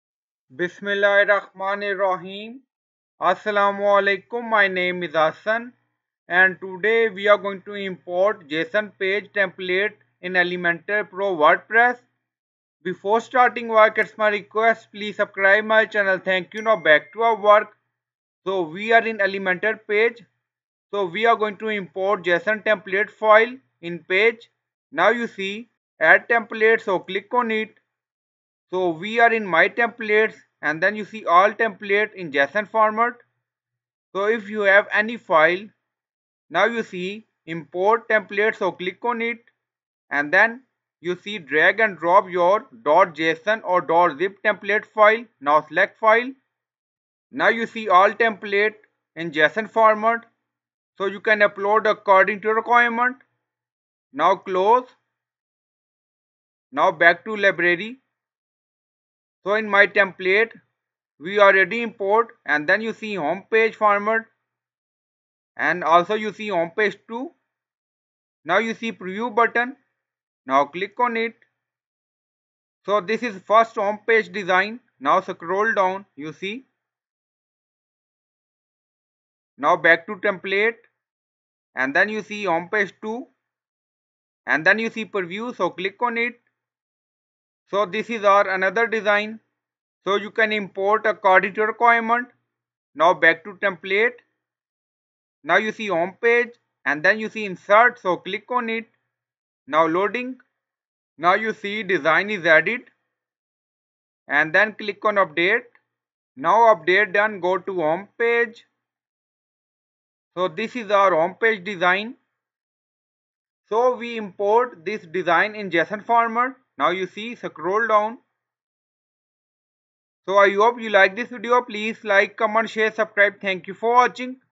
How to Import JSON Page Template Format in Elementor Pro WordPress
In this wordpress tutorial for beginners you will learn how to import or add page template .json file format in elementor pro website page builder plugin in wordpress. So you can import page design in any wordpress website / pages and show design content.